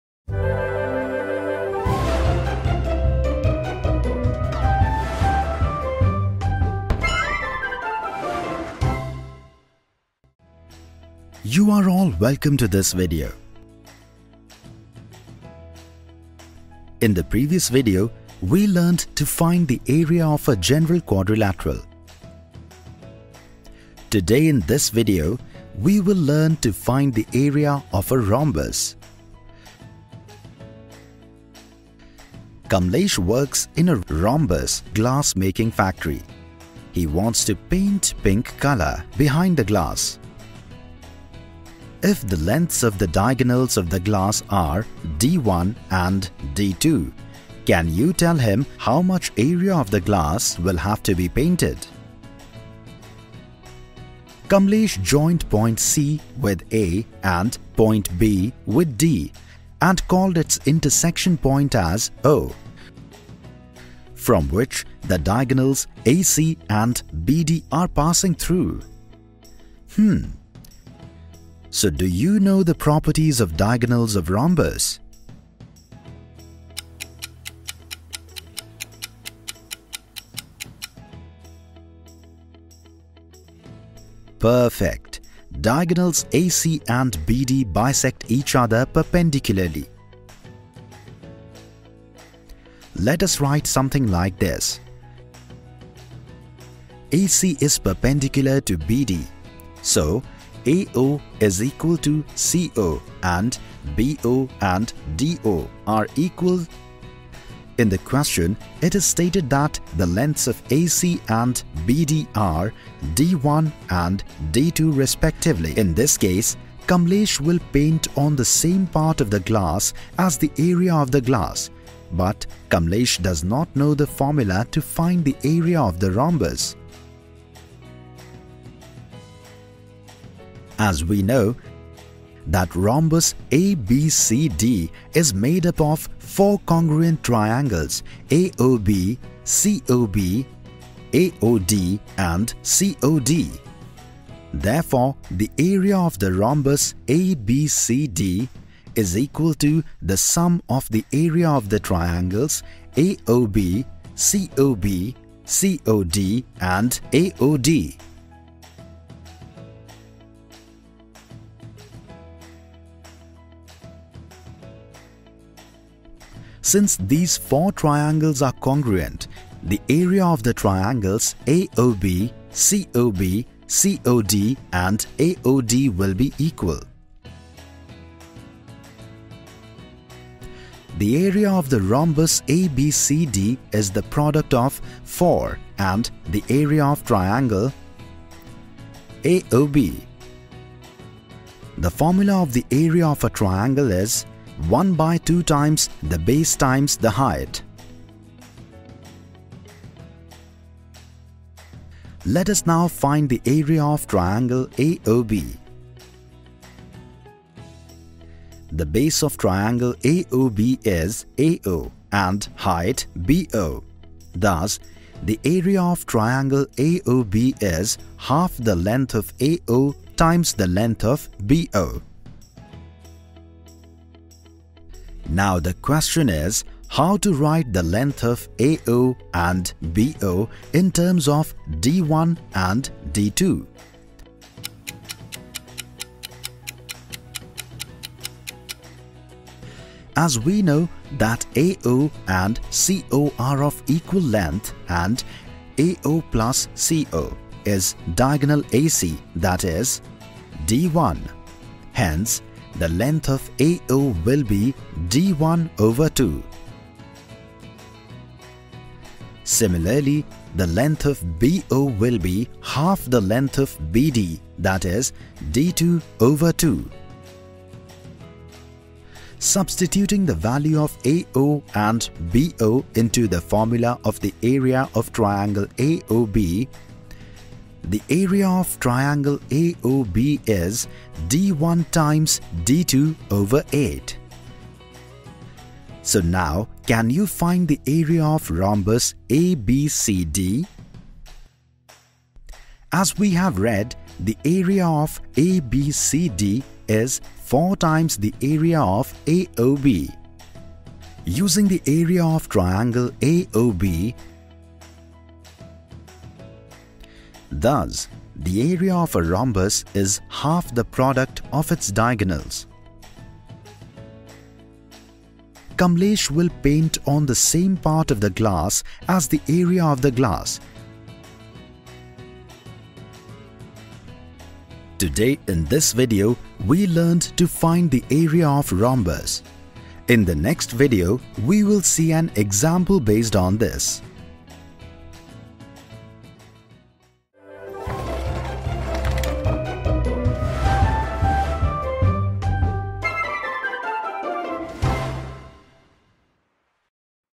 Area of a Rhombus | Part 1/3 | English | Class 8
Area of a Rhombus | Part 1/3 | English | Class 8 | NCERT

Area of rhombus | Perimeter and Area of 2D shapes | Grade 8 | NCERT | Mensuration

Welcome to part 1 of Area of rhombus. In this video, we will discuss the concept of Area of rhombus.